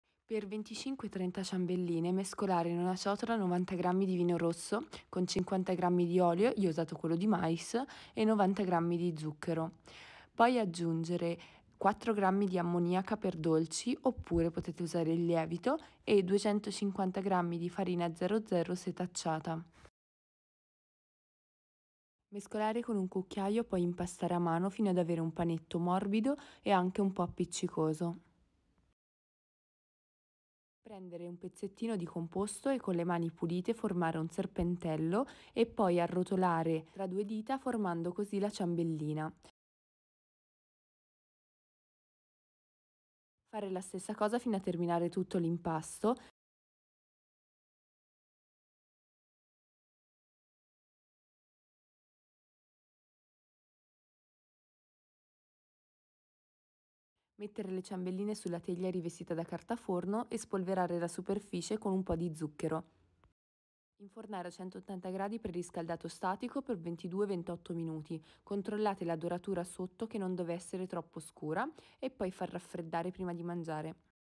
Ciambelline al vino vegane
questi biscottini  sono qualcosa di super delizioso, tranquilli, non vi faranno ubriacare 😂 solo fare una scorpacciata di biscotti 🍪
Il vino gli da un sapore particolare, buonissimo
Potete usare il tipo di vino che preferite, in questo caso io ho usato un Lambrusco amabile, leggermente dolce e frizzantino
Però va bene sia prosecco che spumante che vino fermo. 🔸INGREDIENTI (circa 25-30 biscotti)
90 g vino
50 g olio di semi
90 g zucchero +qb
4 g ammoniaca x biscotti(o lievito)
250 g farina 00
🔸PROCEDIMENTO
Mescolare vino con olio e zucchero. Aggiungere l’ammoniaca e la farina setacciata. Mescolare poi impastare a mano fino ad avere un panetto morbido e leggermente appiccicoso. Con le mani pulite prendere poco impasto , formare un serpentello tra le mani e chiudere formando la ciambellina.
Fare la stessa cosa fino a terminare l’impasto. Mettere l’è ciambelline sulla teglia rivestita da carta forno e spolverare la superficie con un po’ di zucchero.
Infornare a 180 gradi preriscaldato statico per 22-28 min (io 22) controllare la doratura sotto che deve essere dorata ma non troppo. Poi far raffreddare.❤️